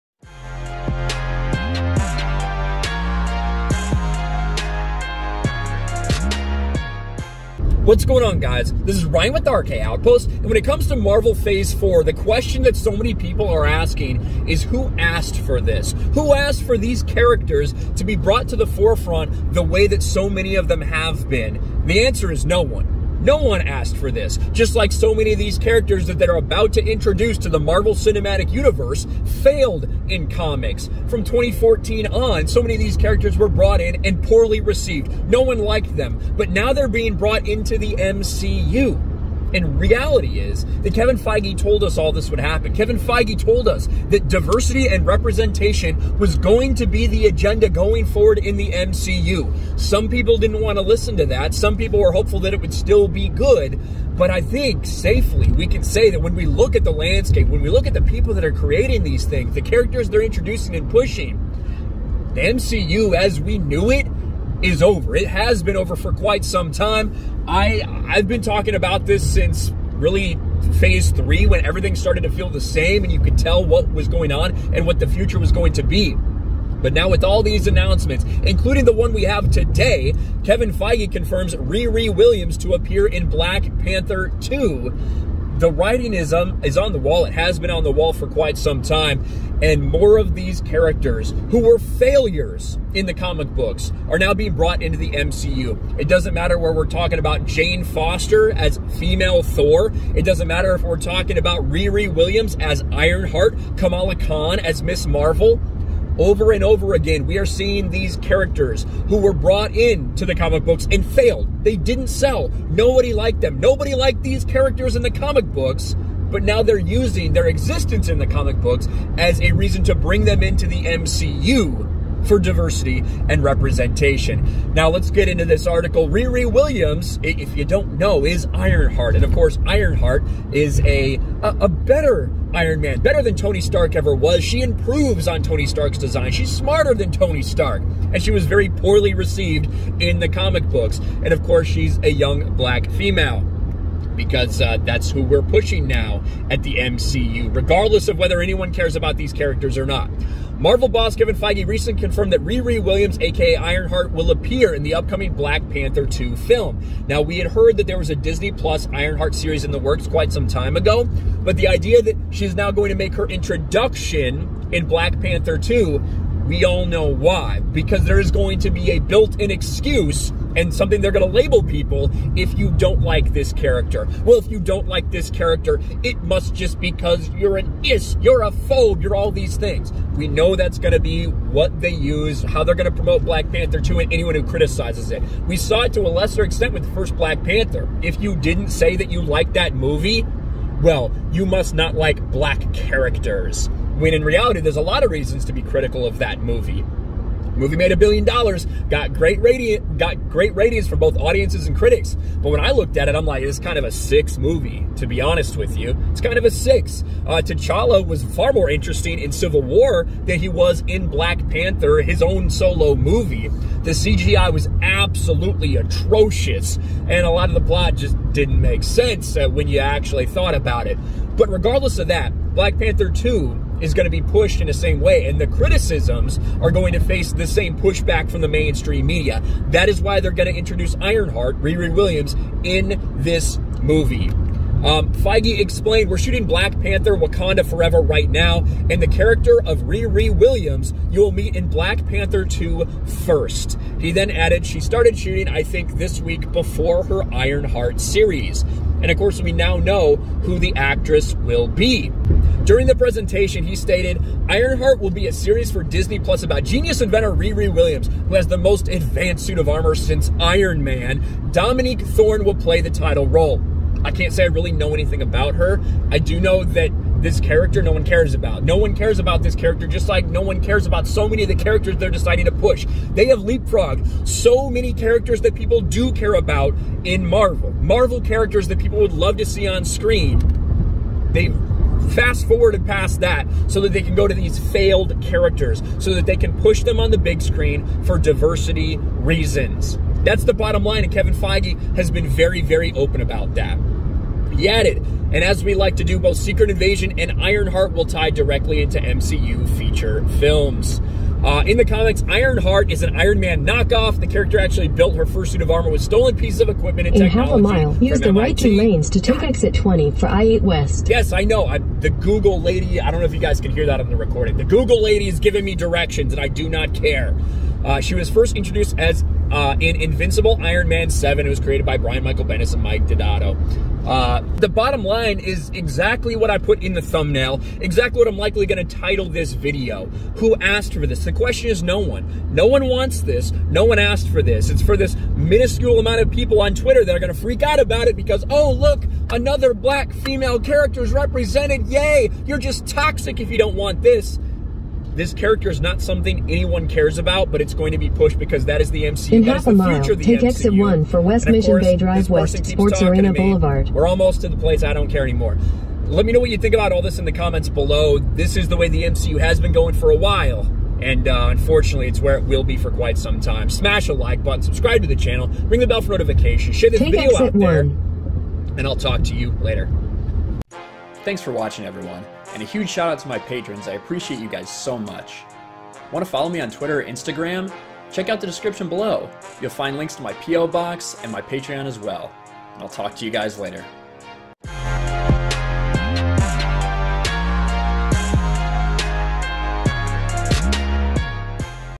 No One Asked For This | Marvel Keeps Pushing Characters That Fans Hate (Ironheart)
Kevin Feige has announced that Riri Williams (Ironheart) will appear in Black Panther 2, yet another character that absolutley no one wants in the MCU.

A HUGE thank you to the following Patrons!:
Mike Jackson -
USDutchkitty –
Iresent Doingthis -
Padawan Level:
Optimus Prune -
R R -